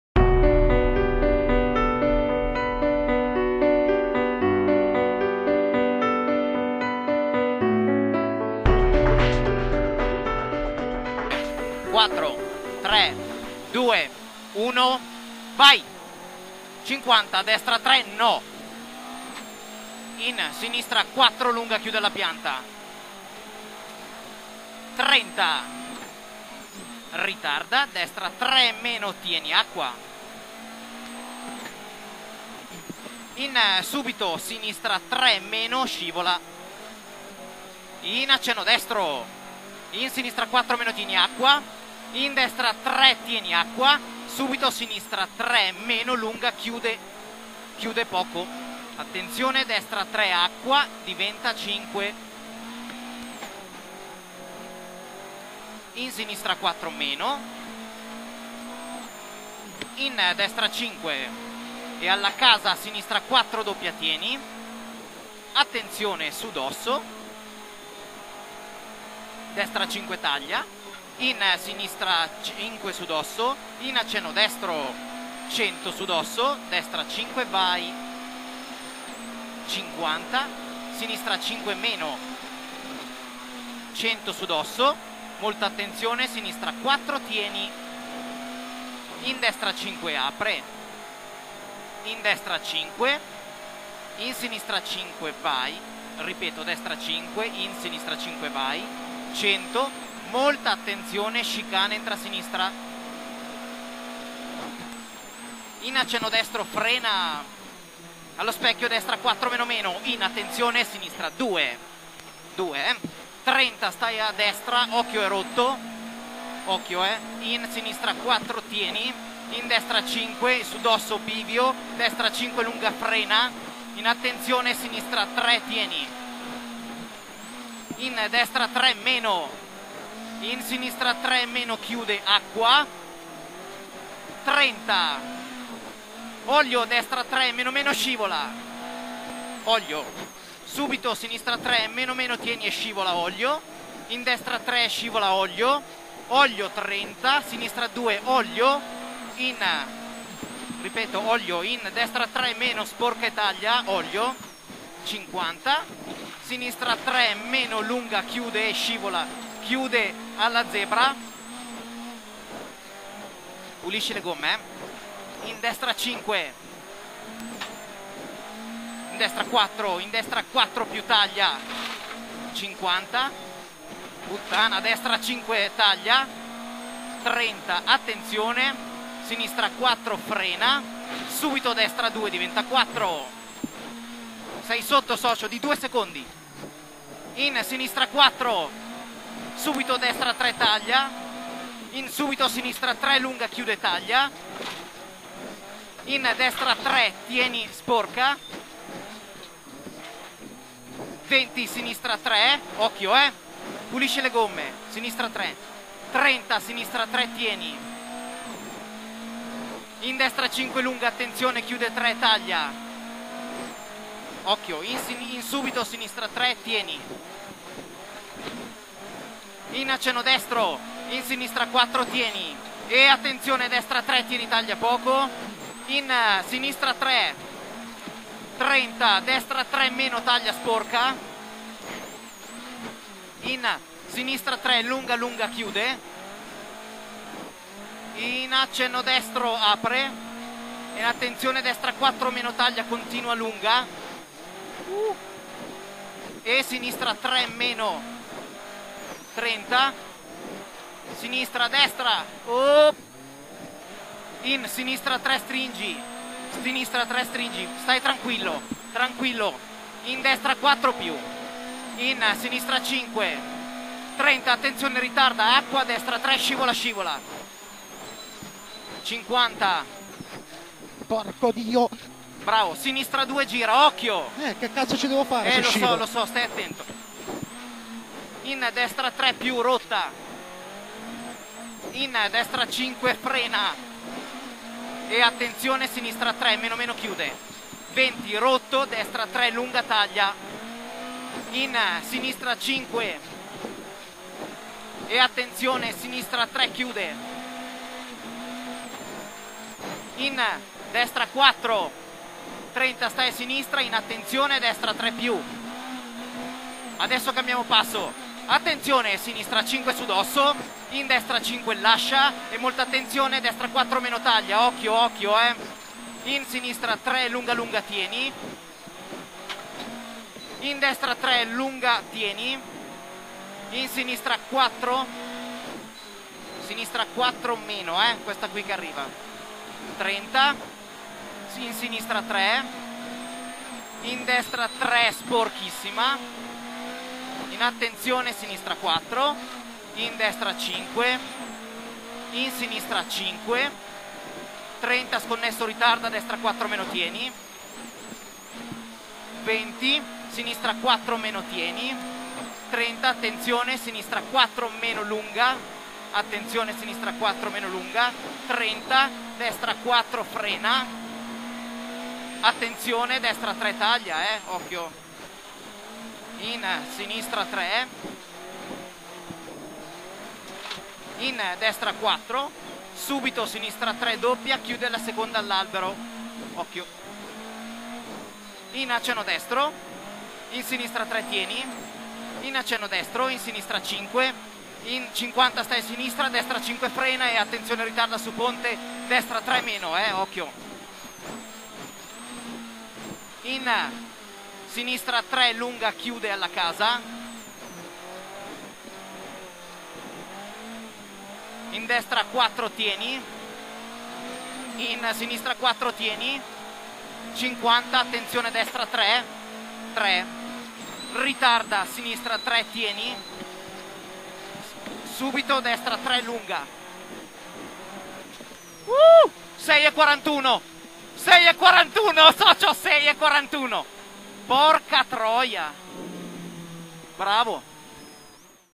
Camera Car SUPER JOLLY - Ronde della Lana 2014 (Bestetti - Nicastri)
1 Ronde della Lana 2014 - Camera Car 1° di 2° Ruote Motrici (Bestetti - Nicastri) Clio A7